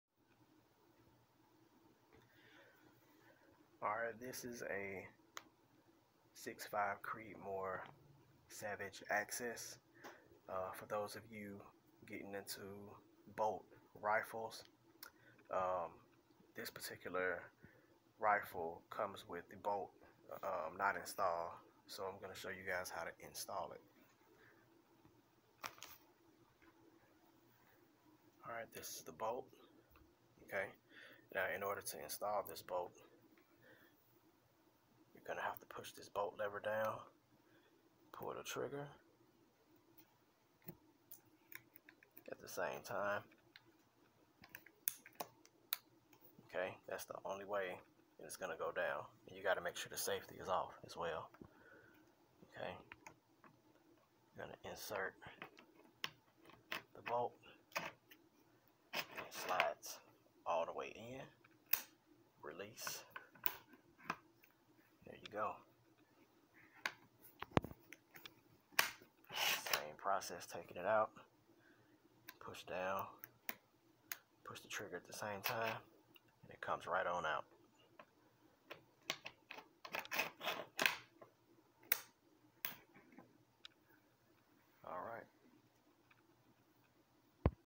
Savage Axis 6.5 Creedmoor bolt install
How to install the bolt on a Savage Axis 6.5 Creedmoor